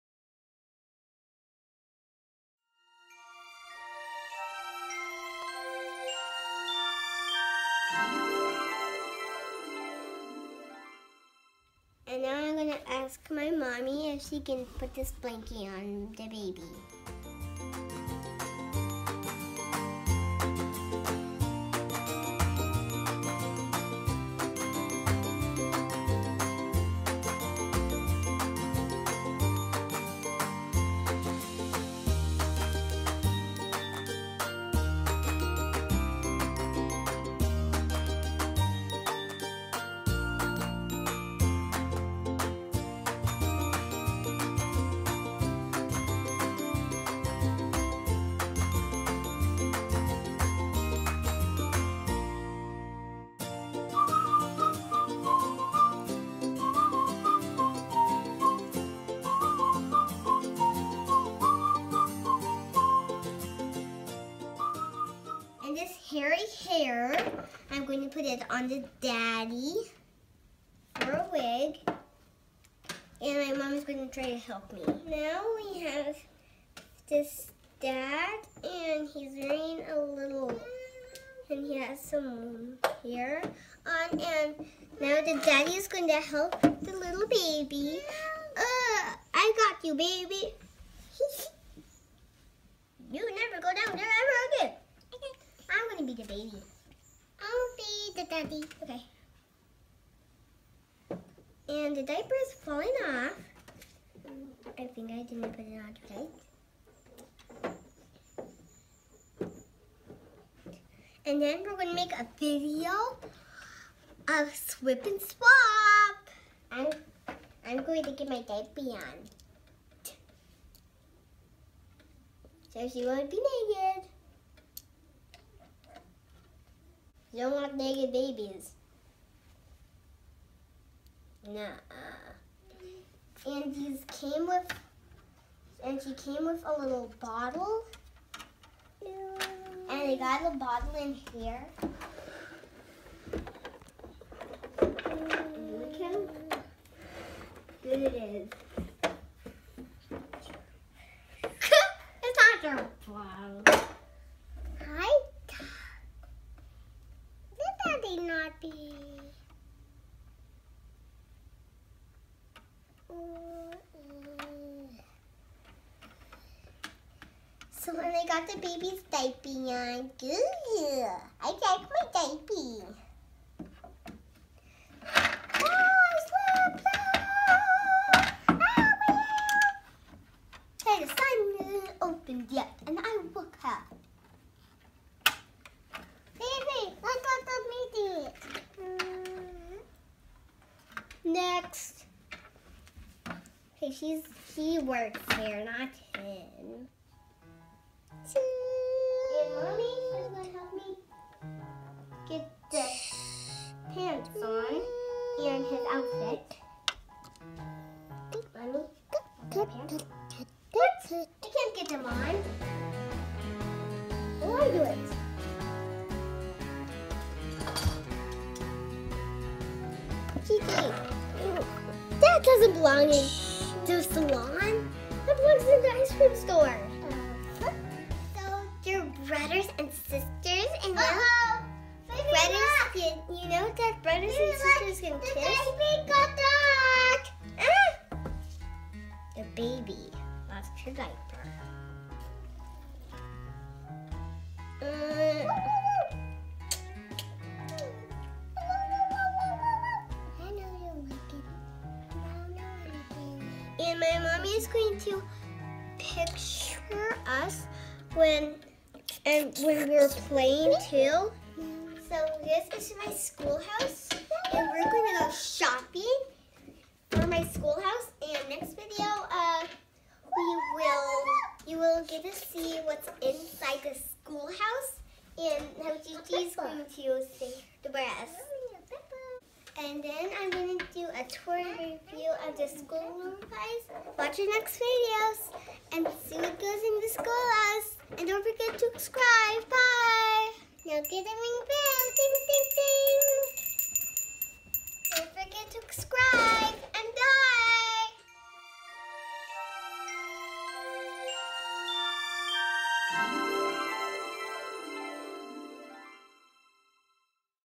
Lil' Woodzeez Honeysuckle Cottage 🏡 Part 2
FiFi reviews and plays with the Honeysuckle Cottage.
Lil' Woodzeez. Fifi and Gigi loves Sugar and Spice. Kids YouTube. Toy reviews. Unboxing.